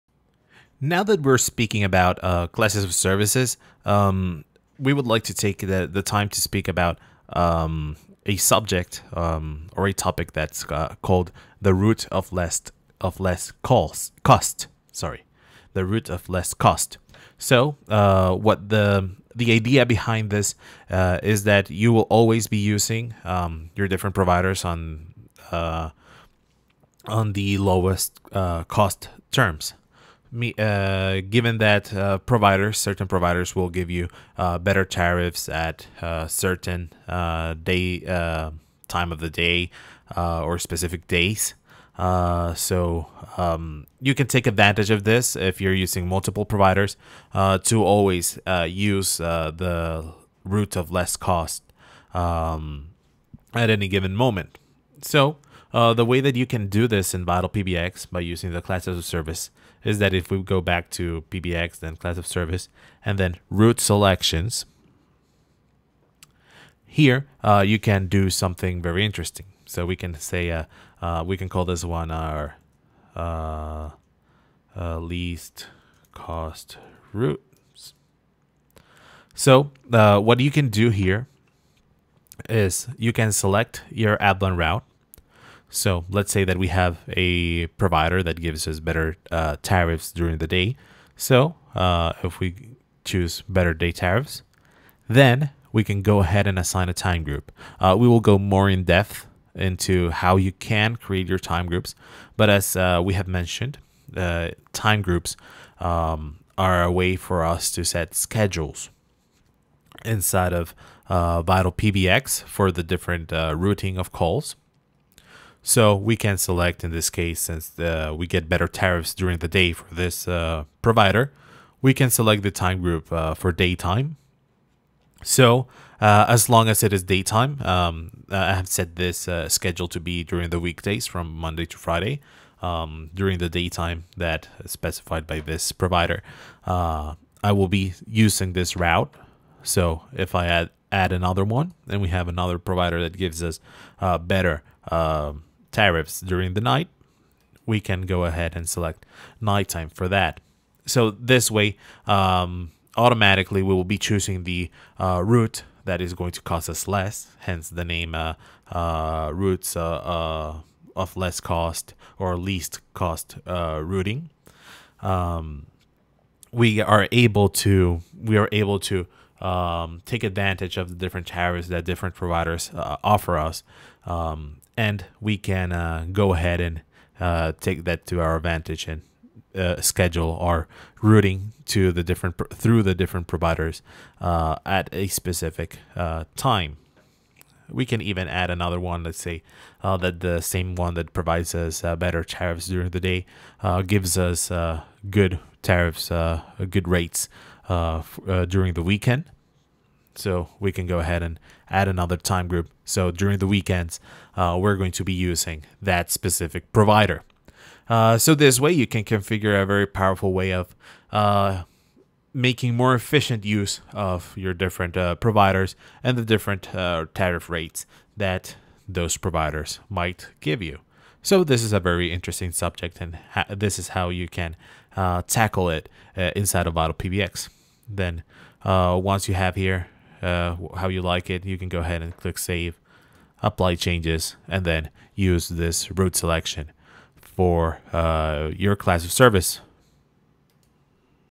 Setting up for Routes of Least Cost in VitalPBX
On this video we show you how you can take advantage of our Route Selections module to have what is called Routes of Least Cost, or Least Cost Routes. This will help you optimize your spending when using multiple VoIP providers.